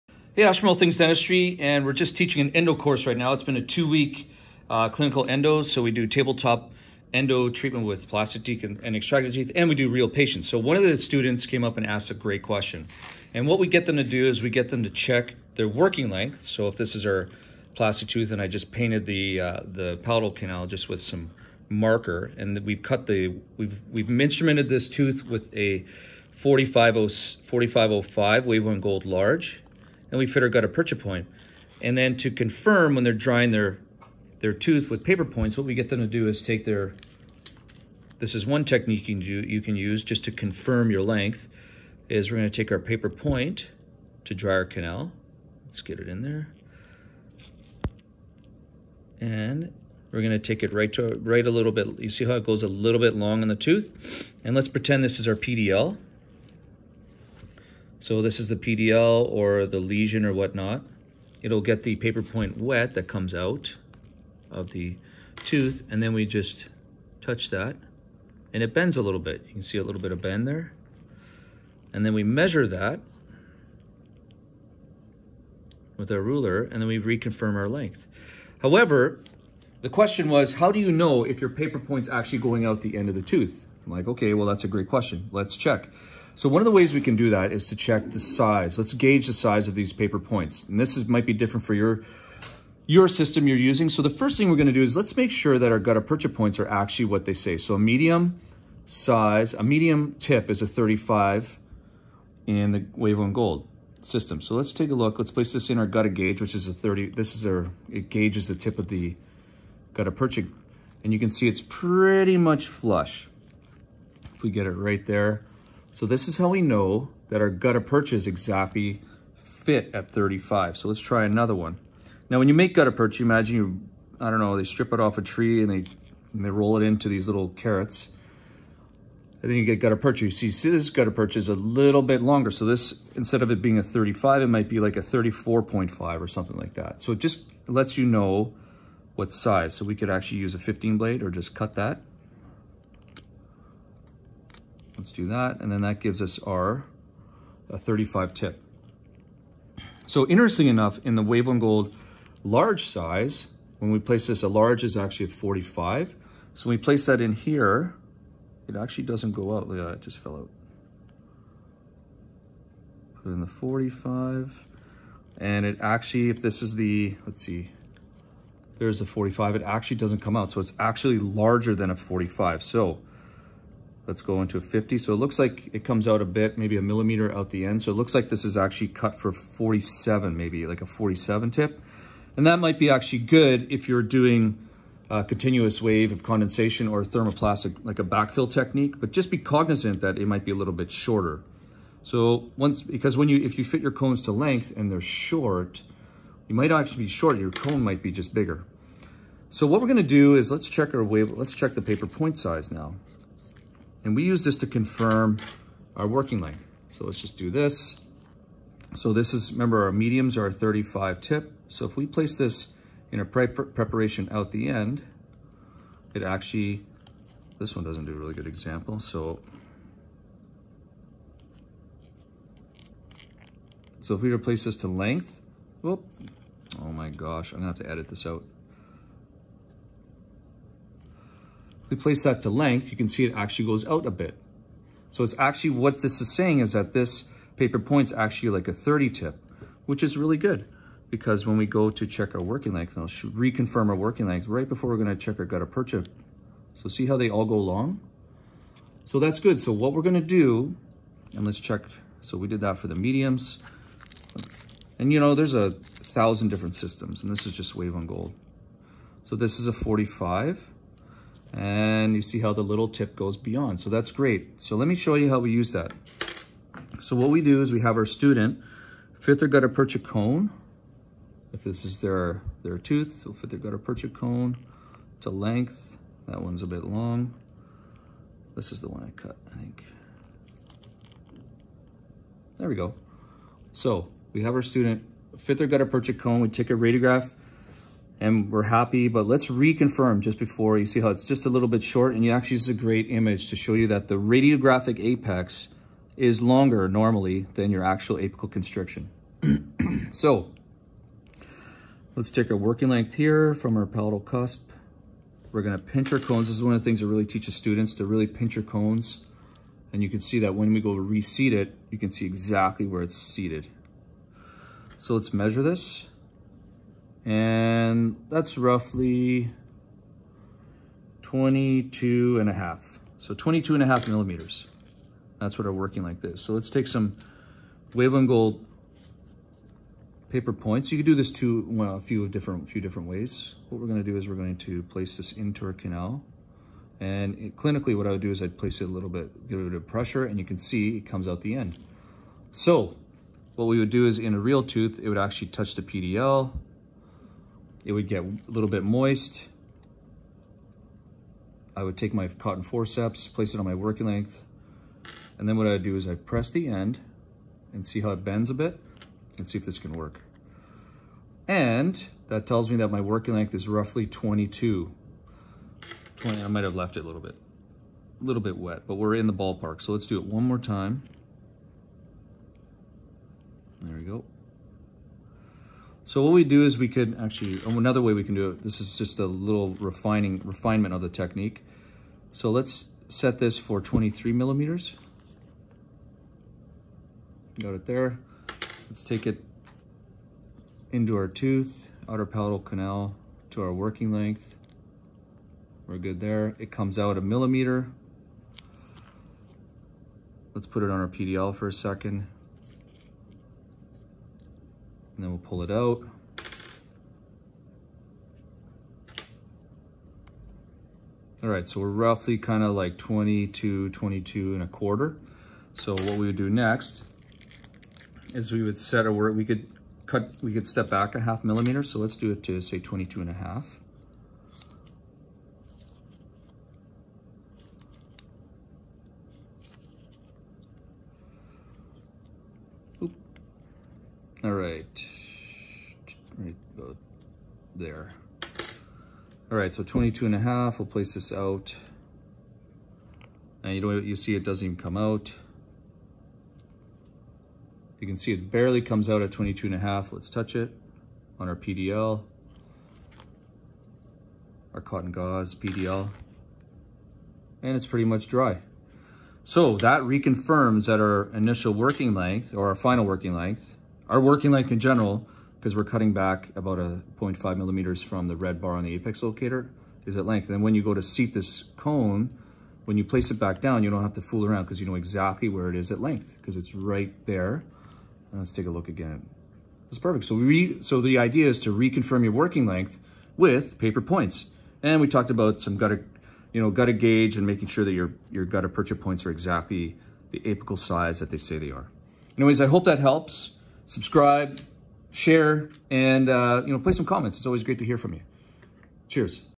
How to Use PaperPoints to Confirm Your Root Canal Working Length
Working Lengths can change as your root canal procedure moves closer to the finish line - and getting a confident working length right before you obturate can be tricky.  Taking a periapical radiograph with a gutta percha cone fitted can help - but often it's tough to see exactly where that gutta percha cone is in a periapical radiograph.  Hence - the paper point confirmation technique.